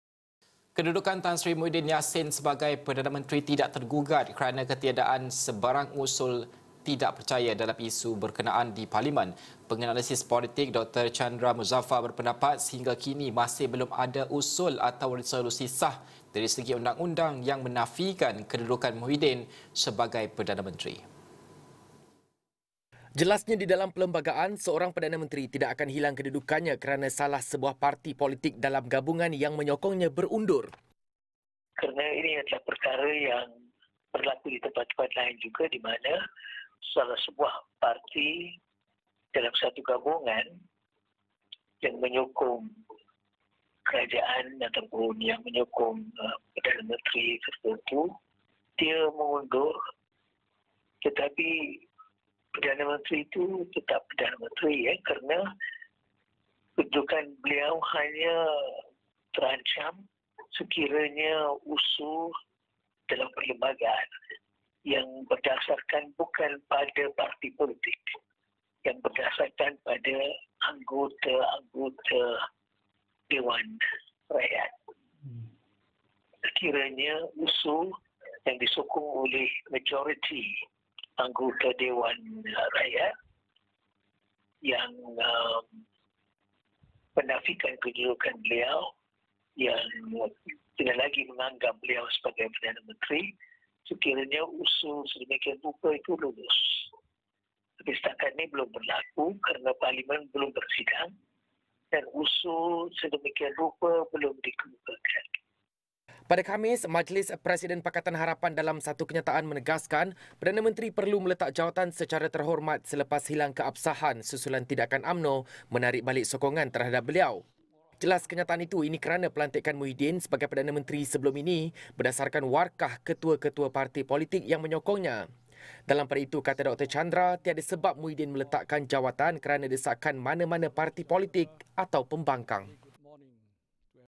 Politik | Kedudukan Muhyiddin tidak tergugat - Penganalisis
Kedudukan Tan Sri Muhyiddin Yassin sebagai perdana menteri tidak tergugat kerana ketiadaan sebarang usul atau kelulusan dalam isu berkenaan di Parlimen, kata penganalisis politik, Dr Chandra Muzaffar.